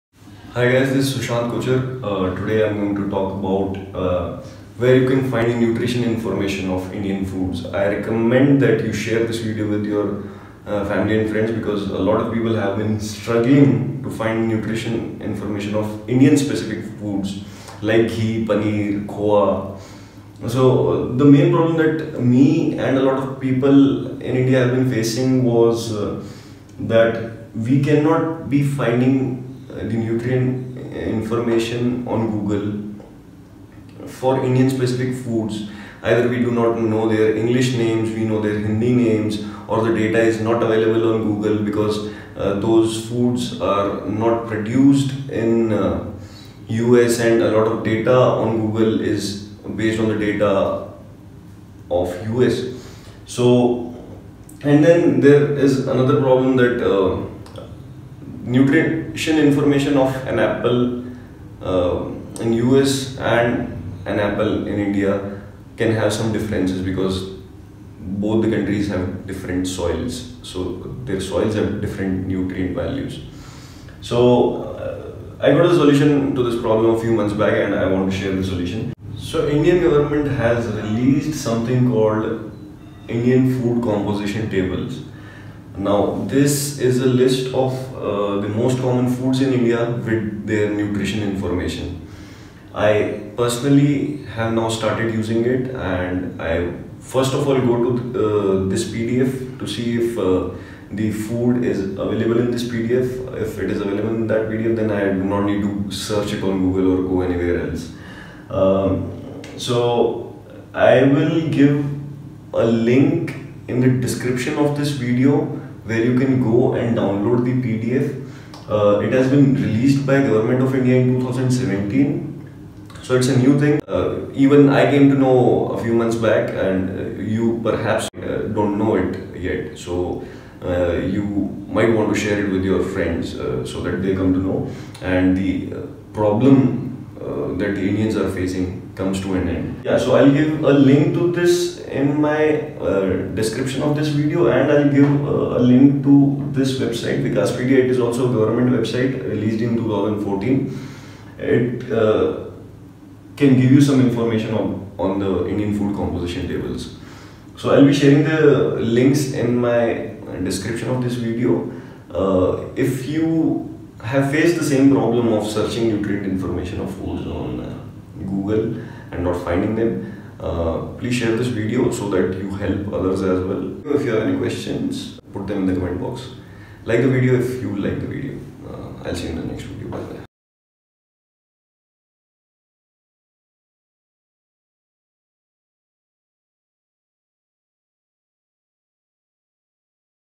Where to find 'Nutrition Information' of Indian Foods? (English)
This video tells where you can find the nutrition facts of Indian food items. Perhaps one of the best ways to find nutrition information of the foods that are native to India is - Indian Food Composition Tables (IFCT). It is English version of the video.